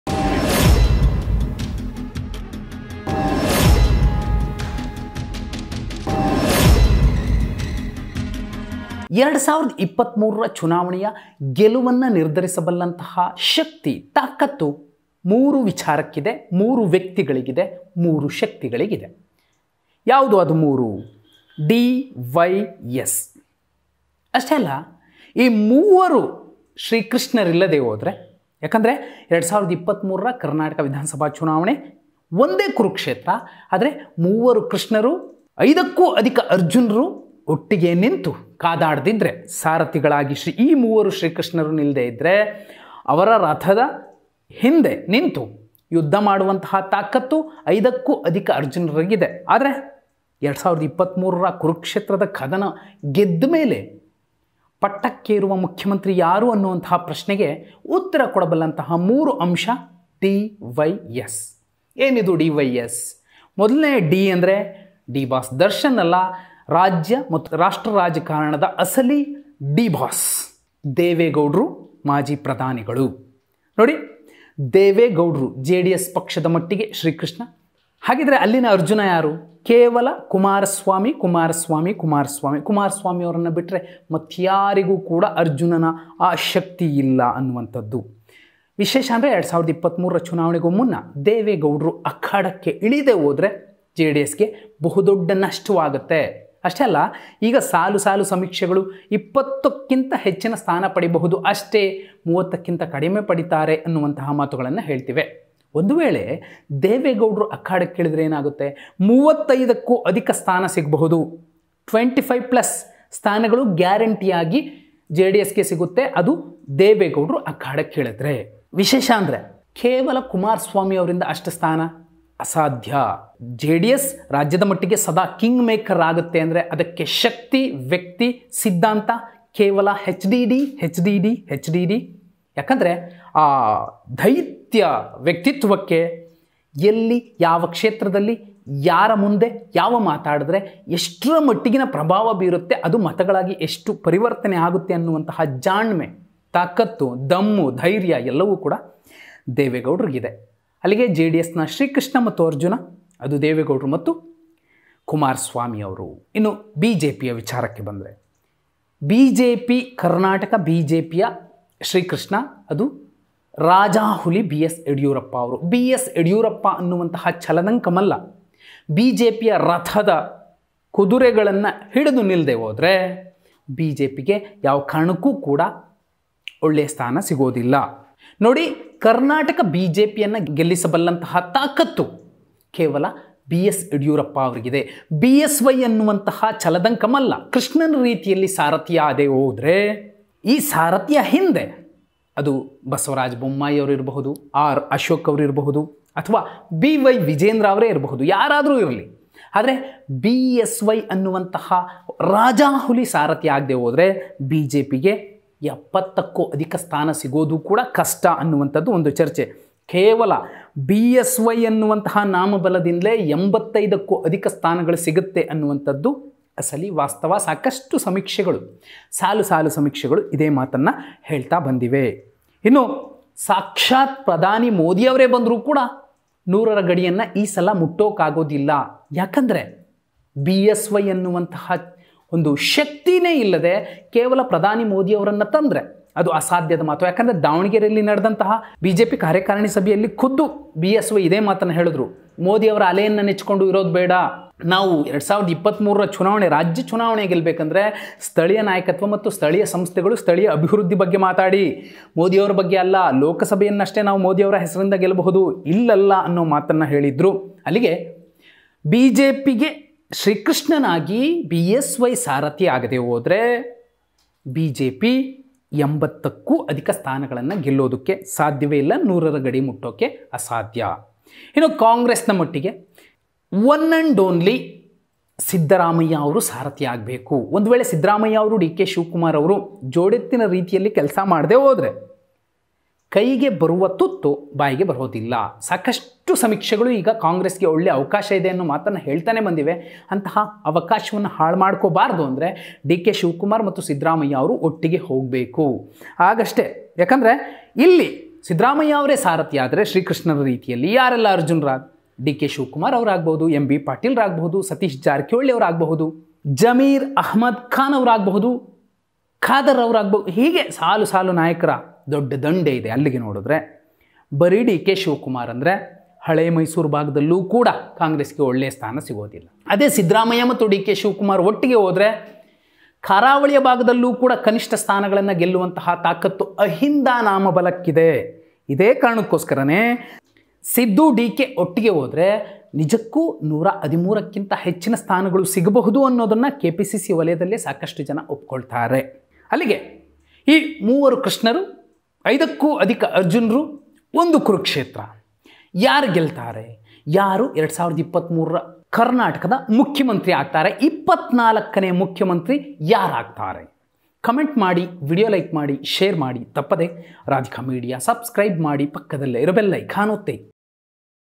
Siddaramaiah BsYediyurappa:3 ಕೃಷ್ಣರು, 3 ಅರ್ಜುನರು ಒಂದೇ ಕುರುಕ್ಷೇತ್ರ!|HD Devegowda |HDK |Radhika media
DYS ಮೇಲೆಯೇ 23ರ ಗೆಲುವು ನಿರ್ಧಾರ?

ಯಾರಾಗ್ತಾರೆ ಕರ್ನಾಟಕದ ಮುಂದಿನ ಸಿಎಂ!?